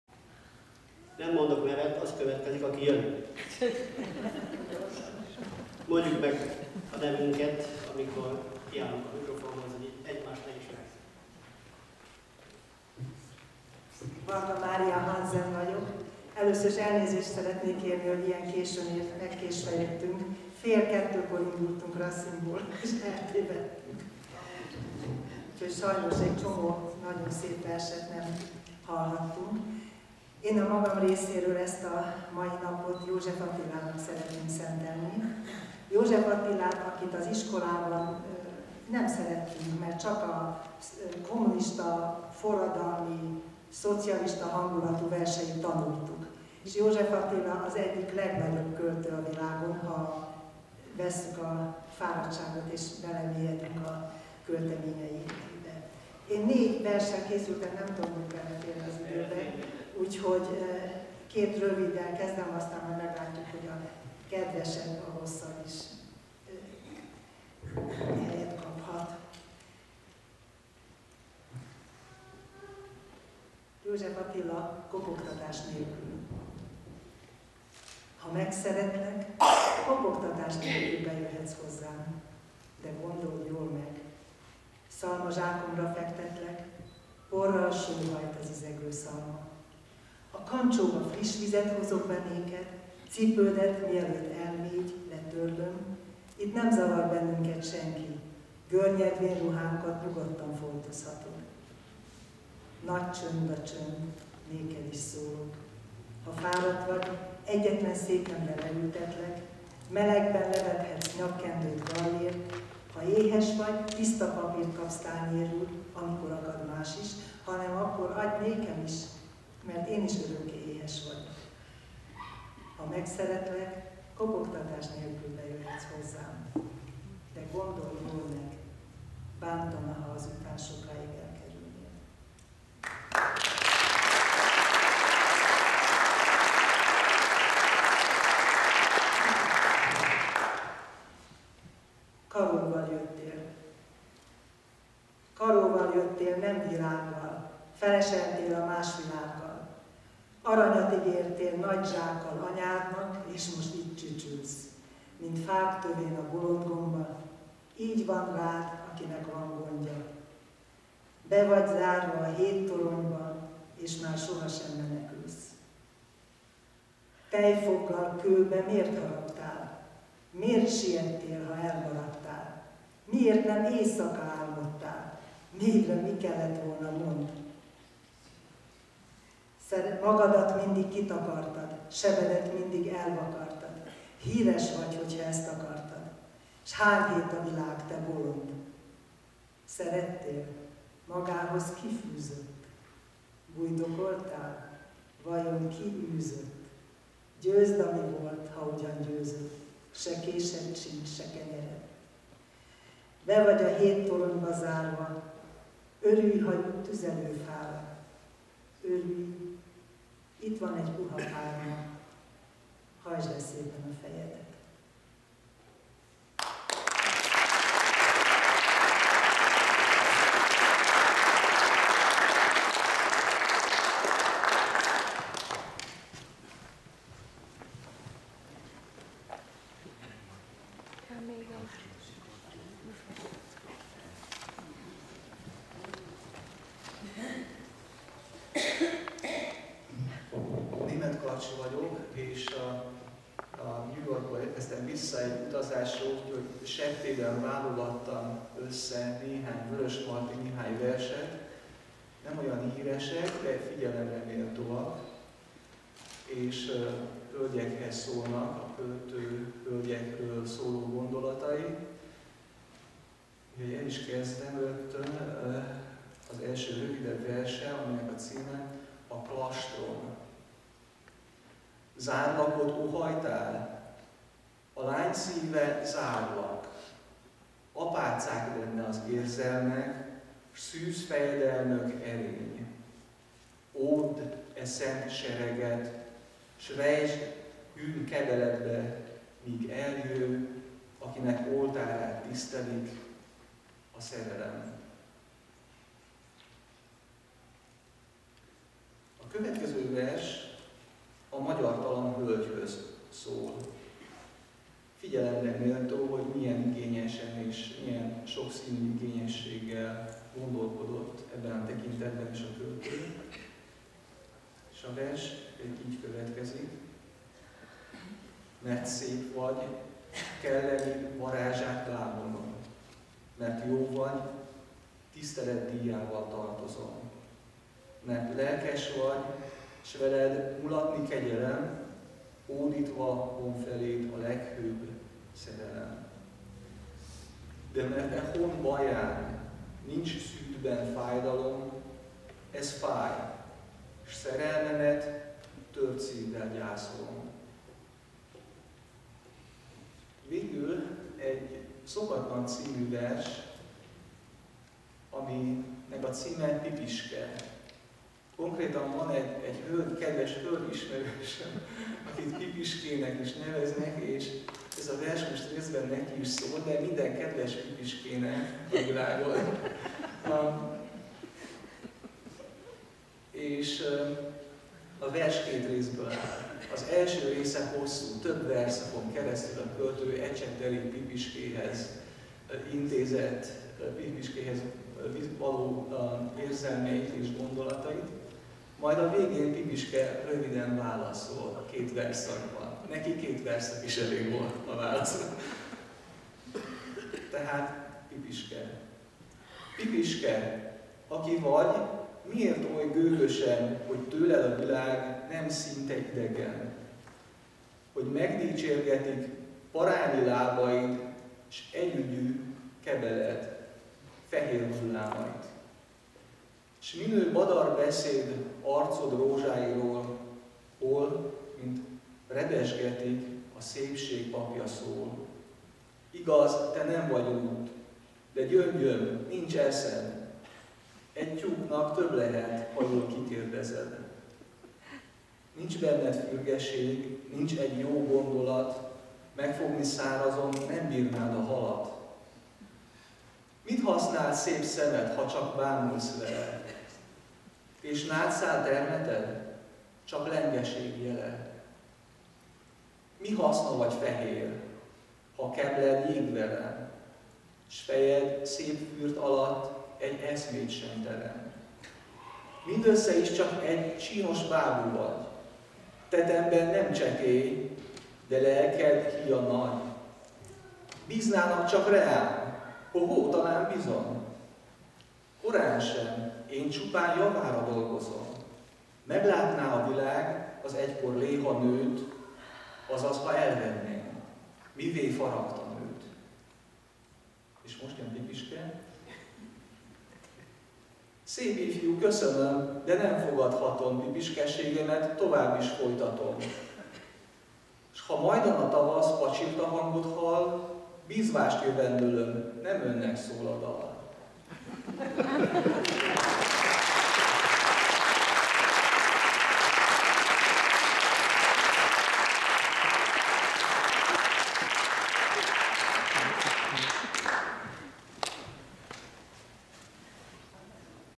Költészet Napja Csikágóban 2014 április,6
Varga Mária és Németh Károly olvasnak fel verseket. A Csikágói Szabad Magyar Református Egyháznál.